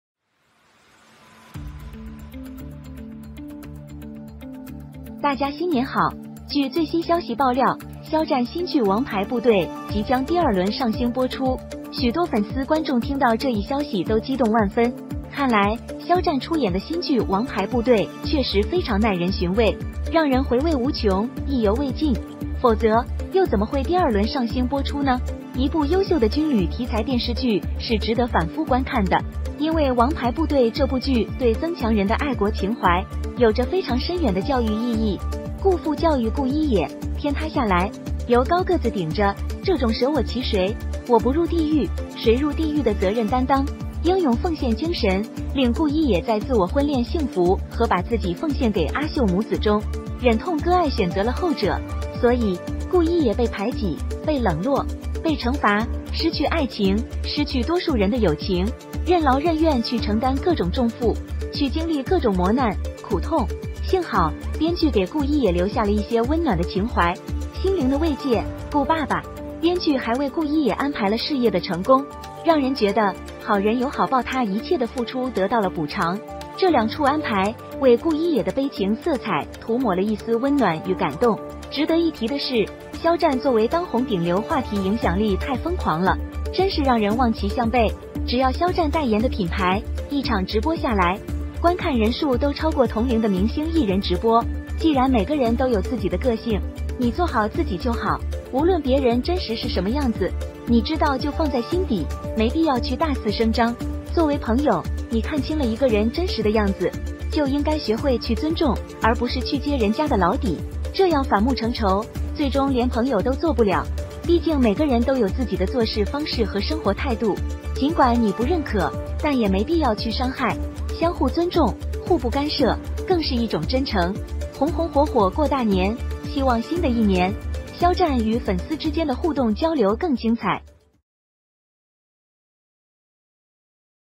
latest news! Xiao Zhan's influence as a top-notch topic is too crazy, is it really worth it?
Good New Year everyone! According to the latest news, the new drama "Ace Force" will soon be broadcast on the second round of stars, many fans and viewers are excited to hear this news. It seems that the new drama "Ace Troop" starring Xiao Zhan is indeed very intriguing, reminiscent of the endless and unending, otherwise, how will the second round of star broadcast? A good military drama is worth watching again and again, because the drama "Ace Force" has a very far-reaching educational significance in enhancing people's patriotic feelings. Xiao Zhao starred in the new drama "Ace Force" to us full of positive energy, Gu Yino from the white metamorphosis into a battle-hardened, calm, missionary soldier, and finally forged a call to come, come to fight, battle will win the ace force. However, after watching the ace unit, Gu Yiyano's life path has been very clear and clear, and Gu Yiyano body tragic beauty, give us a huge shock and impact. Han shrimp is too good at boasting, the rainbow farts smell good and sweet! Gu's father educated Gu Yino, the sky is falling, by the tall man top, this kind of give me who, I do not go to hell, who go to hell responsibility, heroic dedication, so that Gu Yino in self-marital happiness and dedicate themselves to Ah Xiu mother and child, painfully choose the latter. So, Gu Yino was ostracized, left out, punished, lost his love, lost most of his friendships, and worked hard to bear all kinds of burdens, to go through all kinds of trials and tribulations, suffering. In this process, as fans of the audience, we are sad, heartbroken, unfair, sad, what we experienced and felt, is the beauty of Gu Yiyano tragedy. Fortunately, the scriptwriter left some warm feelings to Gu Yino, the comfort of the soul - Gu Dad. Let this sad character have a warm hut in the snow and wind, so as not to be so full of vicissitudes, which also fully alleviates our righteous indignation difficult psychological. The screenwriter also arranged for Gu Yiyano's career success, so that people feel that good people have a good reward for all his efforts have been compensated, these two arrangements, for Gu Yiyano's sad color painted a touch of warmth and touch. It is worth mentioning that Xiao Zhan as a popular top stream topic influence is too crazy, really let people look at its back. As long as Xiao Zhan endorses the brand, a live broadcast down, the number of viewers are more than the same age celebrity artists live. The reason why Xiao war intriguing, people can not stop loving, to a large extent with Xiao war good character and pure three views have an inseparable relationship. Xiao Zhan dares to think, dare to do, dare to break through, never afraid of any gossip, slander and discredit. However, denigrating others will not improve yourself, but will only make you look low. If you can't do it to others, then be hard on yourself, you force yourself to become strong, and no one dares to be hard on you. All the effort, not for others to think you great, but to be able to make yourself look up to yourself in the heart. Don't pay too much attention to what others say about you behind your back, because those who are better than you, simply don't bother to mention you. The denigration, itself is a kind of admiration, as a fan of the audience, no need to be with those small pattern, low-level people in general, because it is not worth meaningless, even if the fight won and what can be? Sometimes, be a little Buddhist, with a little positive, with a little courage, the whole will live more wise and relaxed, calm and relaxed. In this world of life, many times will face many things and choices, will talk is a kind of ability, and can see through not say through is a kind of wisdom. The silence that is appropriate to get along with people is actually more likely to put people at ease. Sometimes more words will be lost, trouble comes from the mouth, a person should know when to say, what not to say, Xiao war this, do a very good job, unlike some people open their mouths to offend people, which pot is not open, single mention which pot, this is not asking for trouble, what is it? Since everyone has their own personality, you do a good job on your own. No matter what others are really like, you know it in the bottom of your heart, there is no need to make a big noise. As a friend, you see a person's true appearance, you should learn to respect, rather than to reveal people's old bottom, so that the antipathy into a feud, and ultimately even friends can not do. After all, everyone has their own way of doing things and attitude to life, although you do not recognize, but there is no need to hurt, mutual respect, do not interfere with each other, but also a sincere. Red hot over the New Year, I hope the new year, Xiao war and fans of interactive communication more exciting!